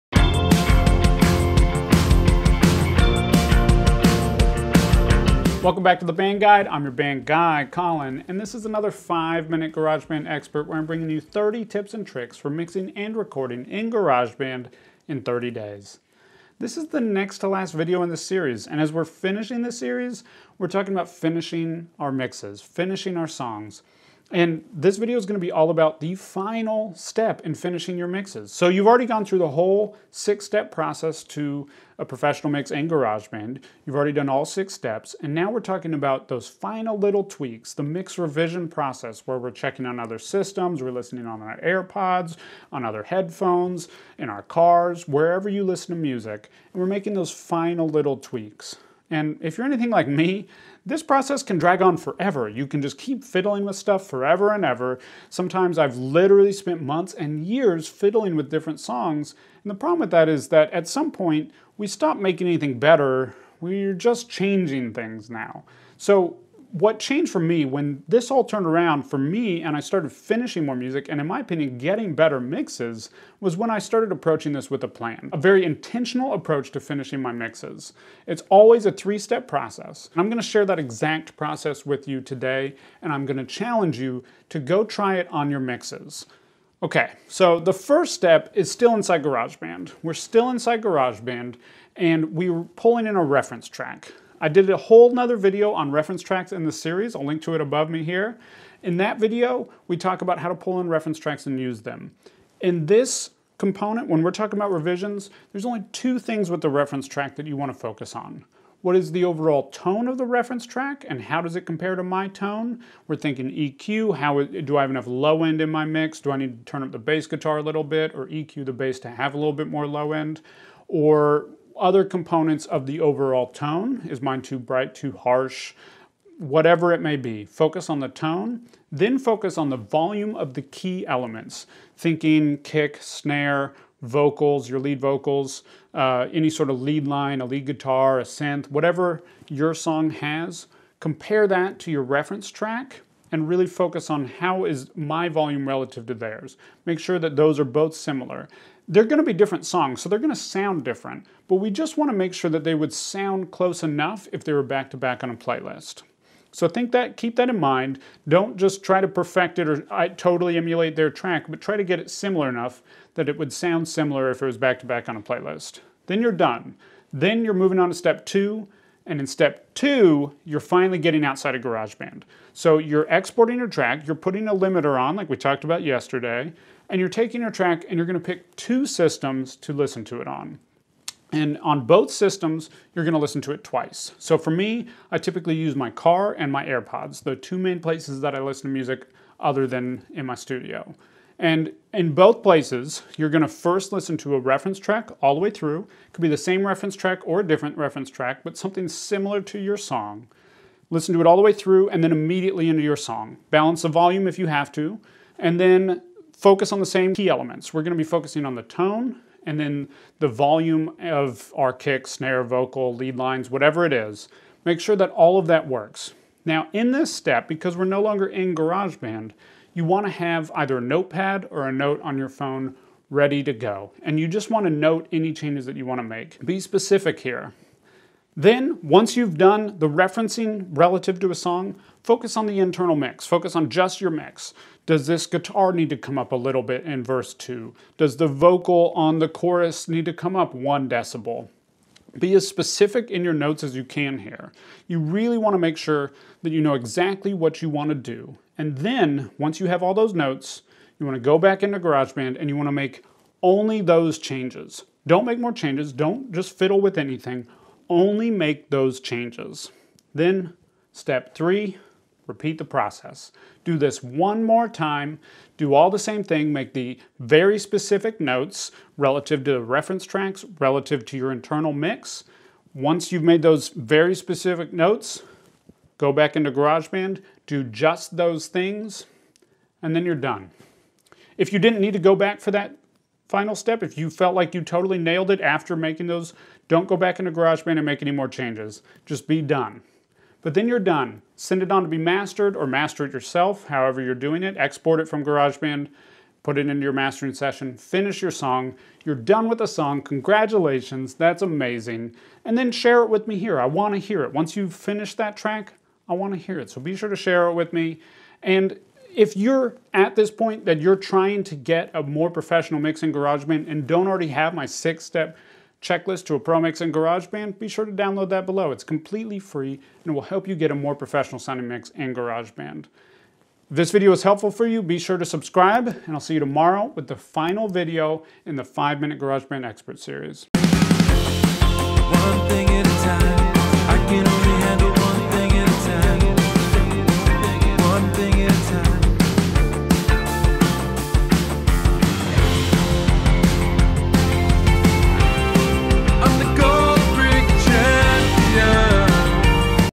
3-Step Mix Revisions | 5-Minute GarageBand Expert
5-Minute GarageBand Expert: Part 29 of 30
Do you spend FOREVER revising your mixes?  How do you know when they are done??  I developed this process years ago and follow it for every mix I finish.  It gives me total confidence that I am prepared to call it DONE.